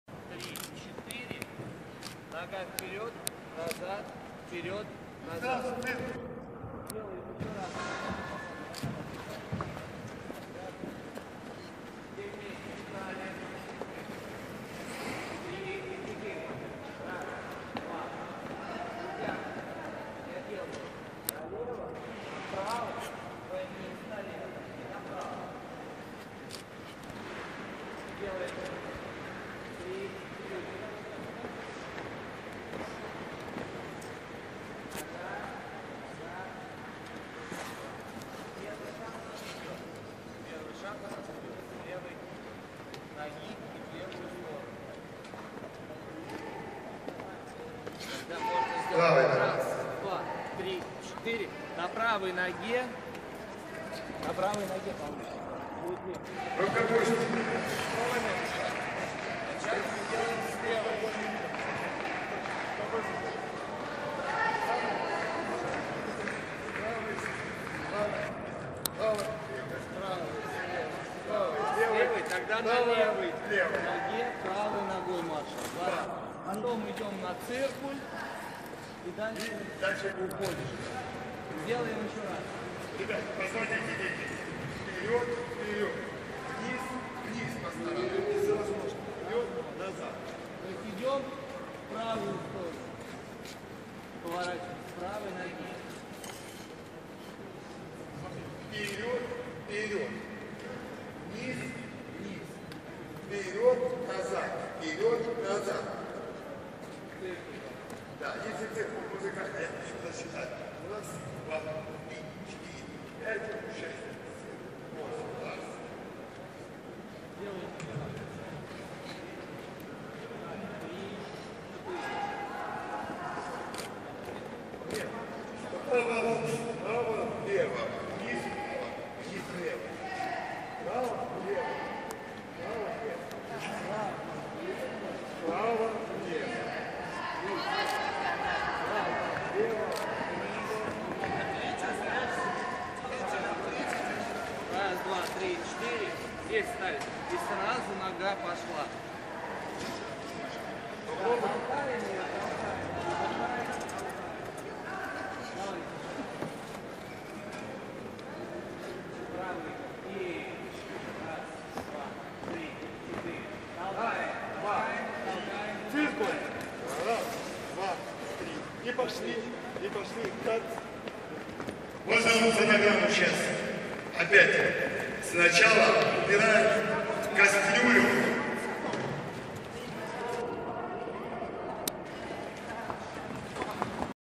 Евгений Плющенко и Давид Авдыш показывают движения поварятам.Снежный Король. Репетиция 25.12.15
Движения поварятам.Хореографы Евгений Плющенко и Давид Авдыш Снежный Король.Репетиция 25 декабря 2015